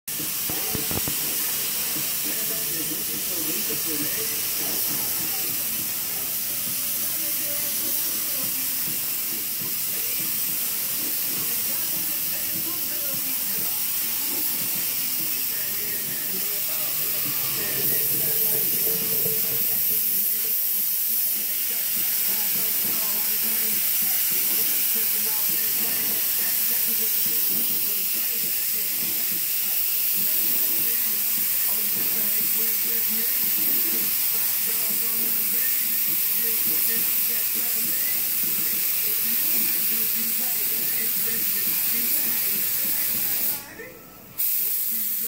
Wet work paint & body spraying tow truck
Wet work paint & body tow truck.
Single stage white.  Too big to fit in the booth.  Had to shoot it in the prep bay.
Bodywork, and, devilbiss, tekna, paint, wett, wet,